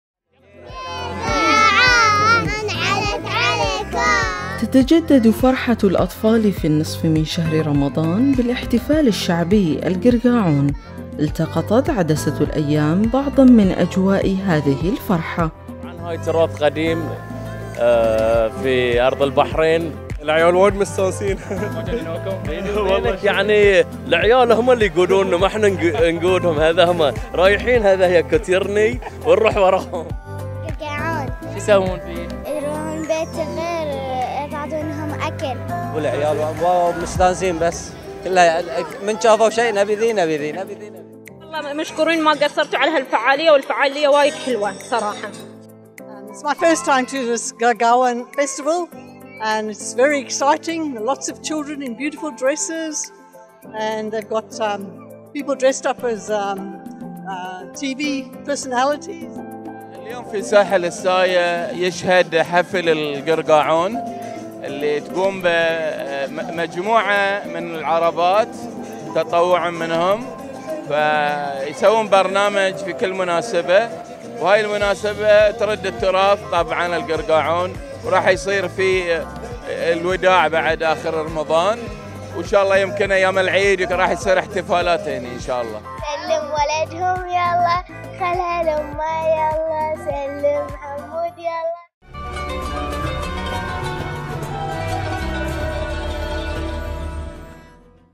«القرقاعون» يبث الفرحة في أرجاء البحرين
تقرير: منصور شاكر
تصوير وتعليق: إيمان الصيرفي